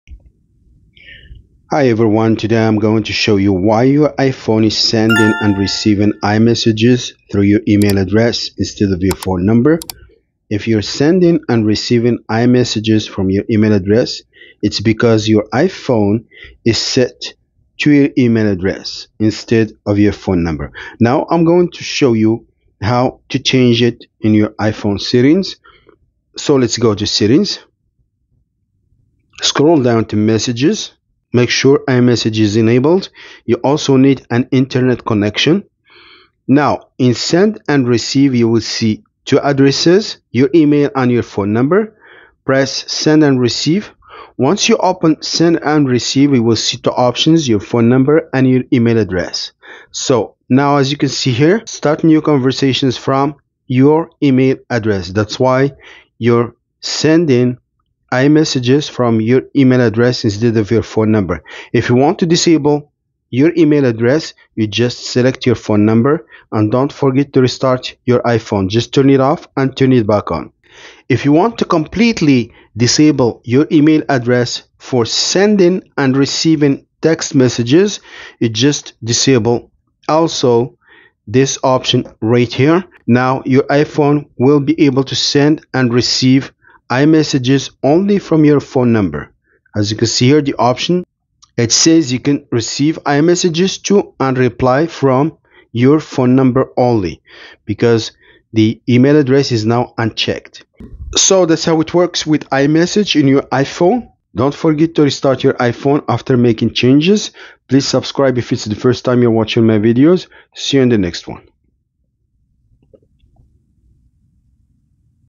Why Is iMessage Sending From Email Instead Of Phone Number, How to Change it
Why Is iMessage Sending From Email Instead Of Phone Number, How to Change it. In this video, we'll walk you through common iMessage problems, including how to fix issues with sending and receiving iMessage from email and how to make changes.